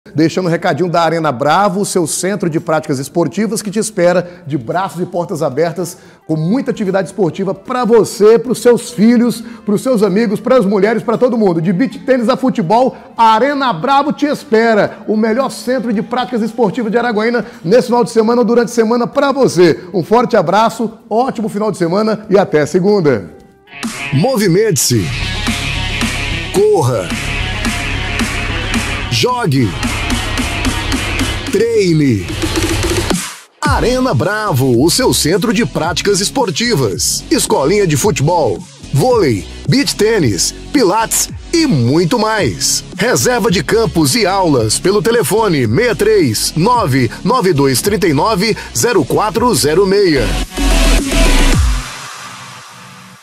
📸 Arena Bravo - O seu centro de práticas esportivas! 🏟️
🌟 Aventure-se conosco e descubra o lugar perfeito para você se divertir e se exercitar! Com uma variedade de atividades esportivas, a Arena Bravo oferece opções para todos os gostos e idades. 🙌

⚽️ Entre em campo com a nossa renomada escolinha de futebol, onde os talentos são lapidados e os sonhos ganham vida. Nossos treinadores dedicados estão prontos para ajudá-lo a alcançar o seu potencial máximo no esporte mais amado do mundo. ⚽️

🏐 Se você é apaixonado por vôlei, aqui é o seu lugar! Nossas quadras modernas estão prontas para receber jogos emocionantes e desafiantes. Junte-se a nós e faça parte de uma comunidade vibrante de amantes desse esporte incrível. 🏐

🎾 Para os fãs de beach tennis, temos as quadras perfeitas. Desfrute de partidas divertidas e desafiadoras em um ambiente descontraído e ensolarado. Solte seu swing e faça novas amizades enquanto pratica esse esporte emocionante. 🌴

💪 Se você busca fortalecer seu corpo e mente, nosso estúdio de Pilates é o local ideal. Com instrutores qualificados e equipamentos modernos, oferecemos aulas que vão ajudá-lo a melhorar a postura, a flexibilidade e a consciência corporal. Venha experimentar os benefícios do Pilates! 💪

🌟 Na Arena Bravo, temos muito mais a oferecer! Nossa infraestrutura moderna, instrutores experientes e ambiente acolhedor garantem que você tenha uma experiência esportiva inesquecível. Venha nos visitar e descubra o seu próximo desafio esportivo! 🎉

Estamos ansiosos para recebê-lo na Arena Bravo e ajudá-lo a alcançar seus objetivos esportivos. Junte-se a nós e faça parte dessa comunidade de entusiastas do esporte! 💪🏆